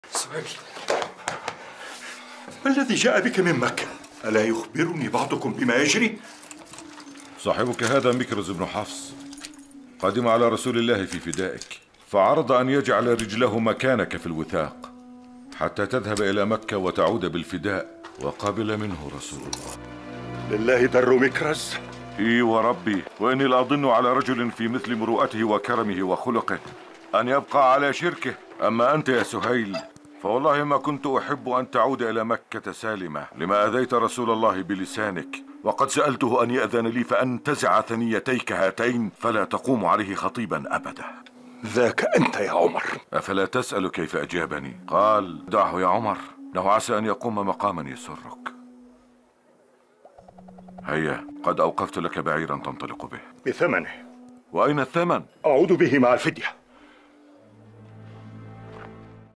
سهيل بن عمرو والفاروق عمر | دعه لعله يقف موقفا يسرك
عندما فك أسر سهيل بن عمرو (خطيب قريش)
وقول النبي لعمر بن الخطاب: دعه لعله يقف موقفا يسرك

لو عجبك الفيديو لا تنسانا من اللايك و الشير
ما تنساش تشترك في القناة